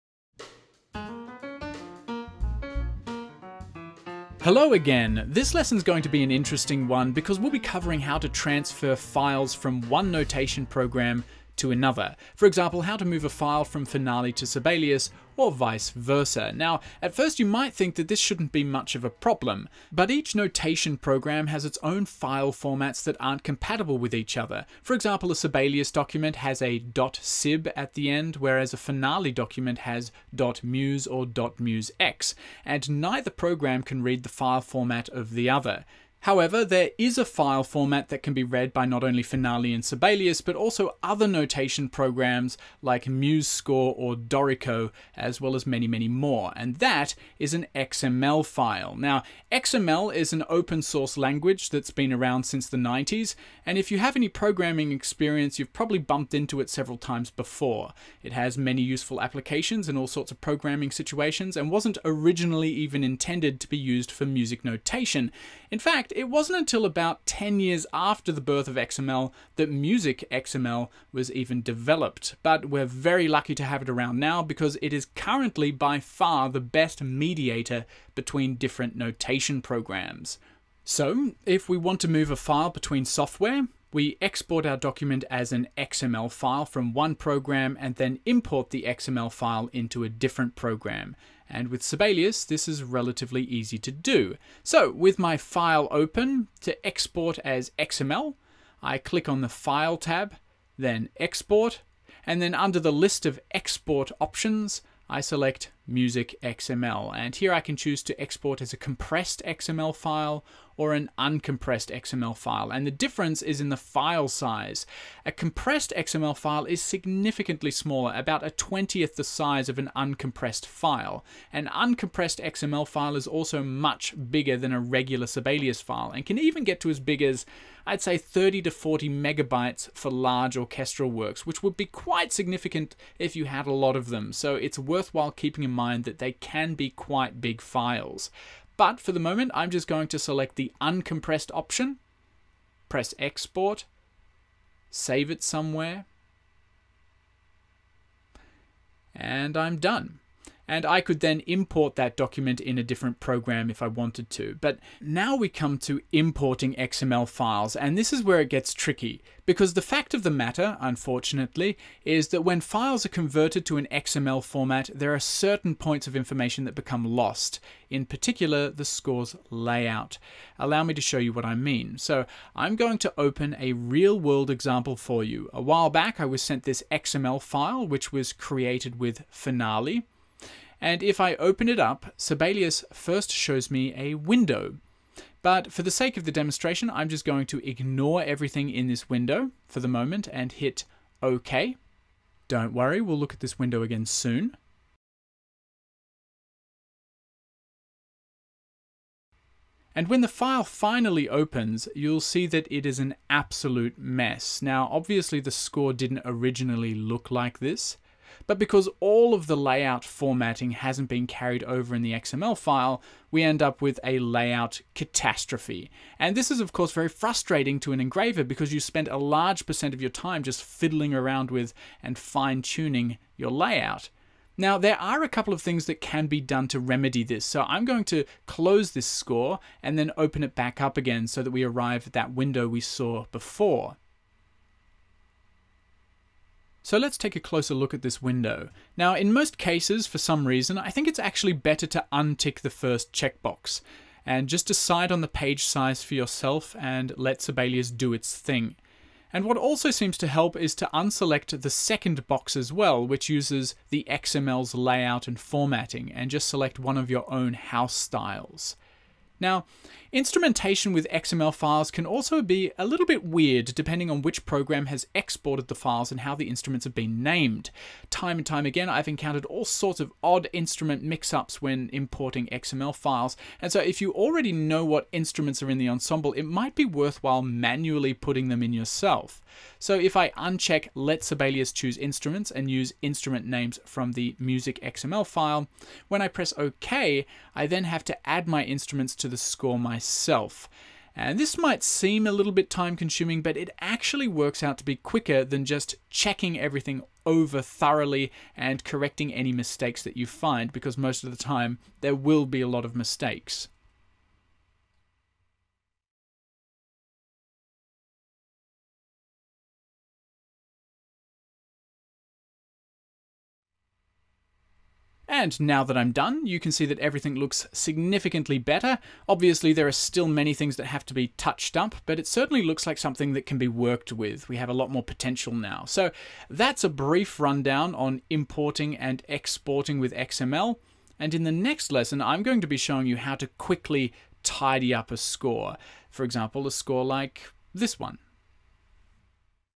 Sibelius XML Import Export
Transfer files between music notation programs like Finale, Sibelius, MuseScore, and Dorico. We demonstrate how to export a file as .xml in Sibelius and discuss the difference between compressed and uncompressed .xml files. Importing .xml files can be challenging as layout information can be lost, but we provide tips on how to improve the process, such as manually selecting page size, applying your own house styles, and adding instruments yourself.

- Originally created in 2019 as part of the course "Take Your Sibelius Skills to the Next Level".